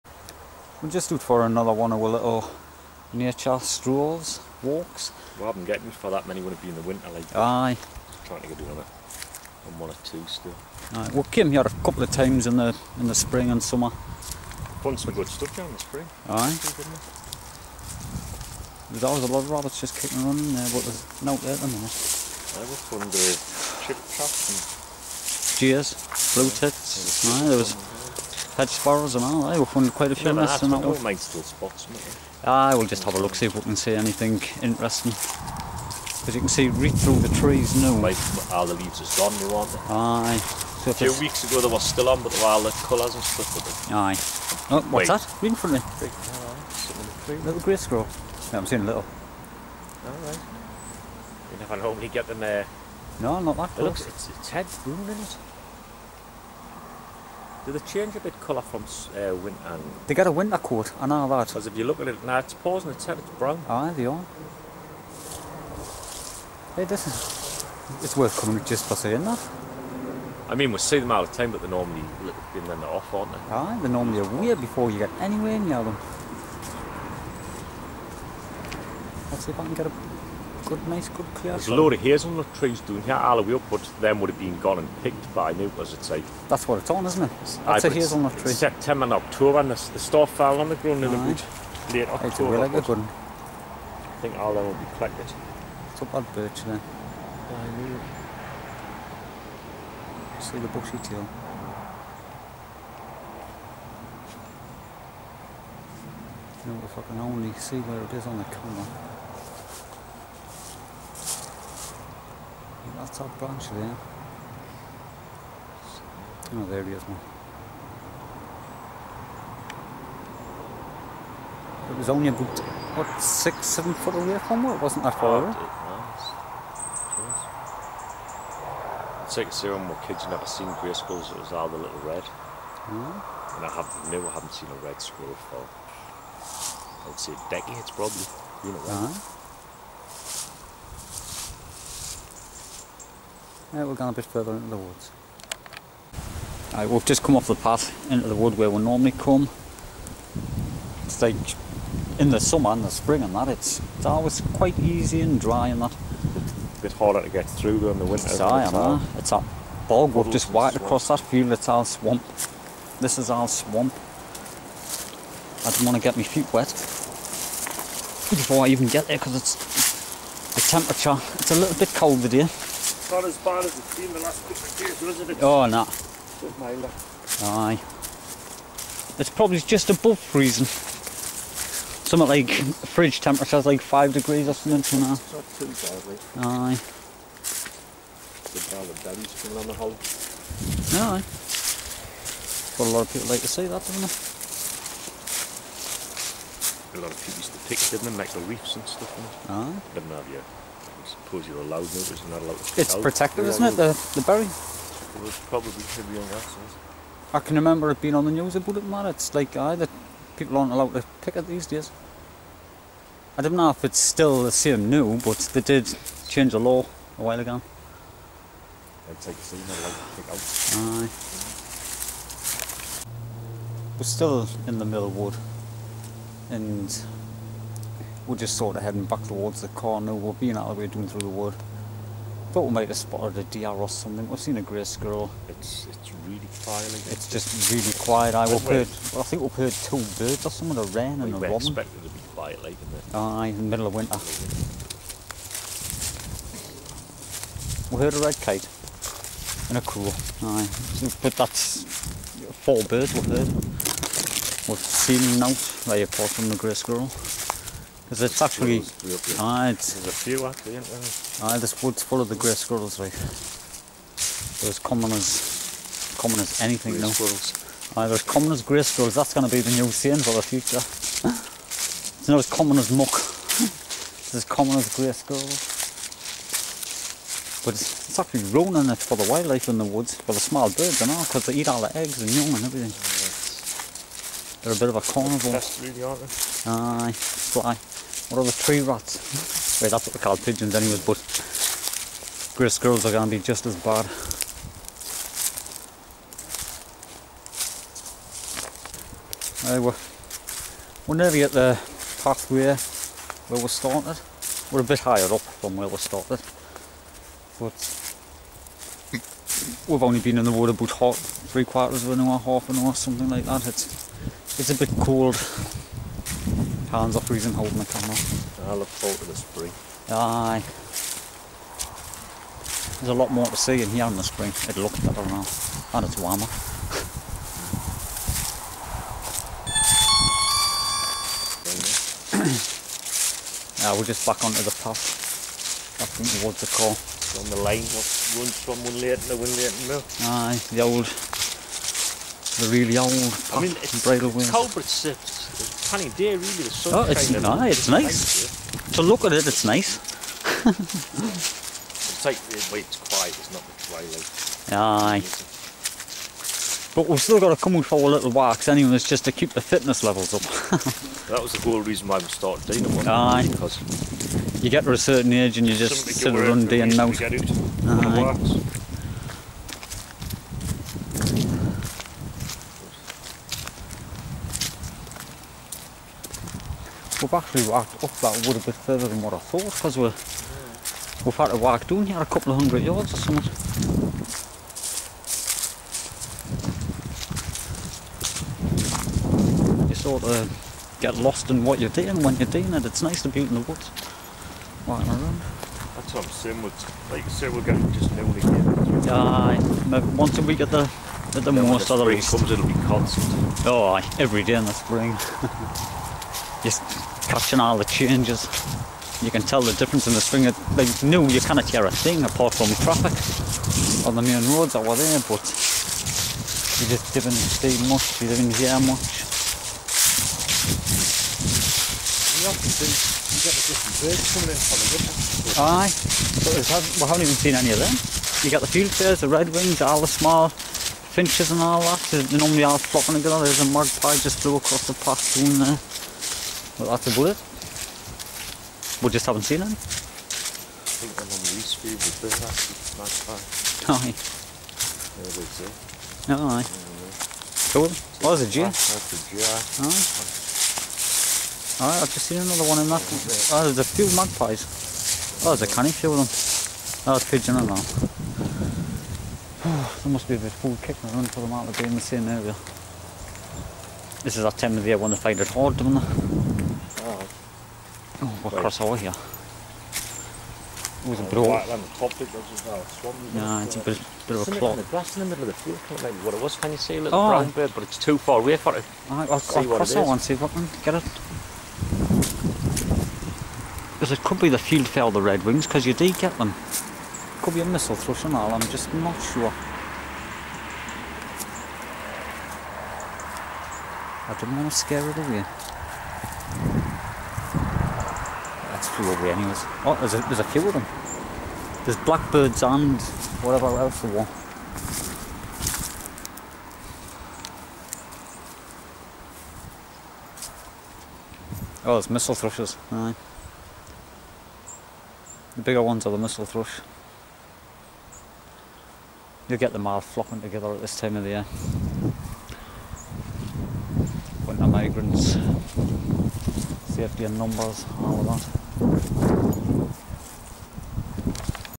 winter walk, grey squirrel and birds
close video of a grey squirrel and some birds